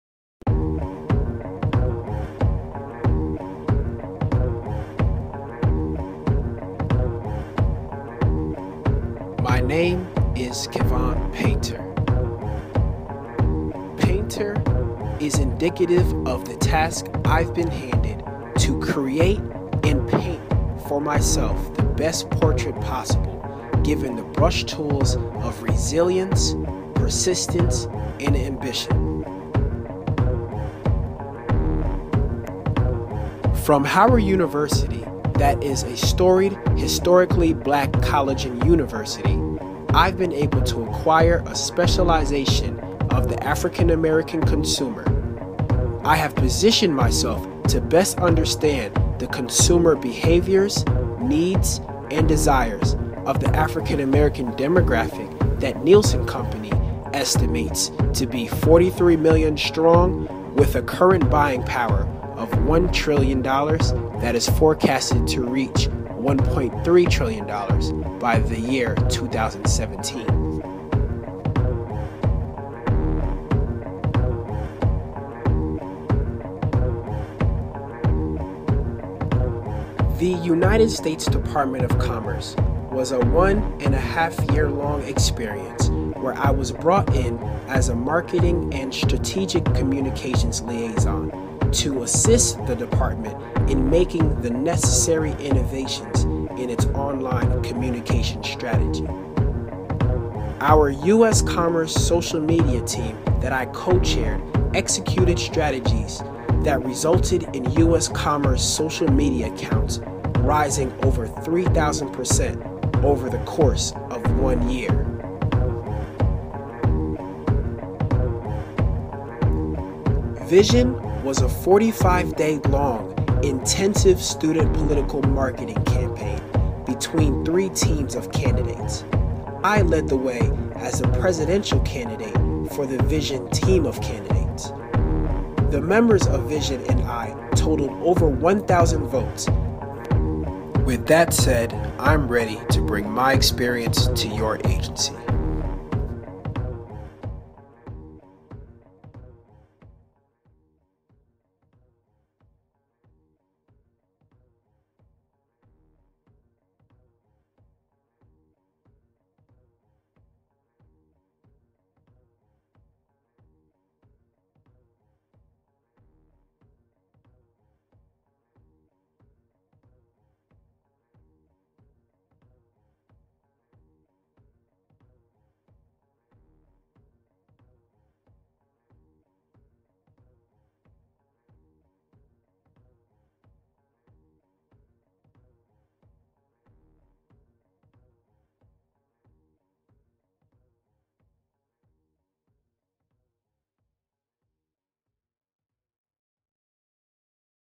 InternAtTranslation
My summer's dream is to unleash my youth's boundless energy on projects of cultural importance and Translation LLC provides a unique opportunity for myself to contribute as an active participant, curator of culture, and creative marketing.

enter: About.me/kevon.paynter

Tweet @PulseofCulture with InternAtTranslation tell them to you say to "pick Kevon" and why!